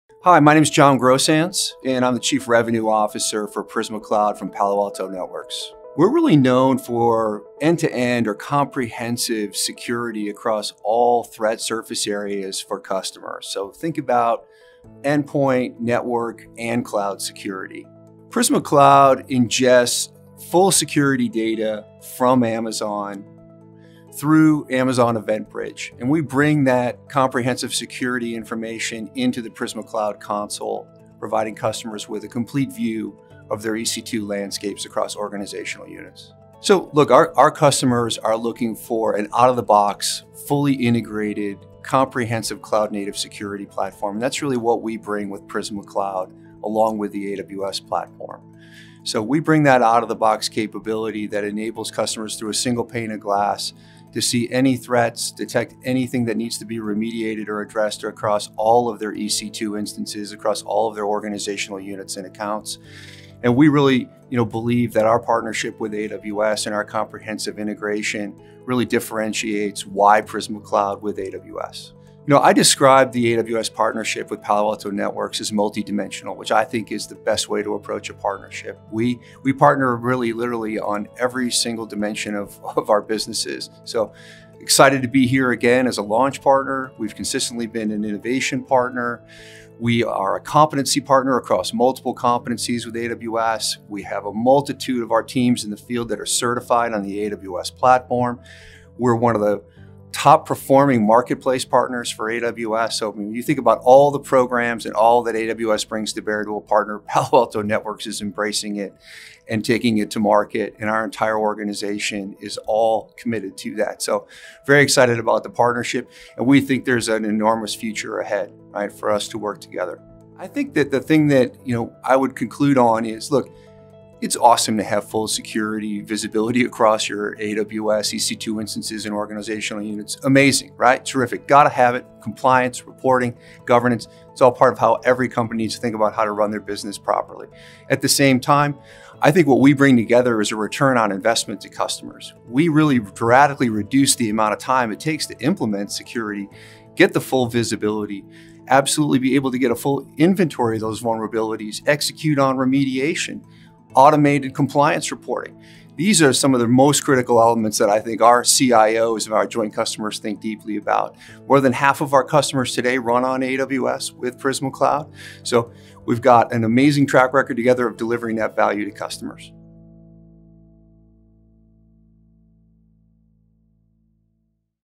Delivering Enhanced Inspector Assessments with Prisma Cloud and AWS
Prisma Cloud by Palo Alto Networks, together with Amazon Web Services (AWS), enhances cloud security at any scale with vulnerability assessments from across AWS using the latest Amazon Inspector.

Learn more about how Prisma Cloud supports the latest Amazon Inspector for enhanced security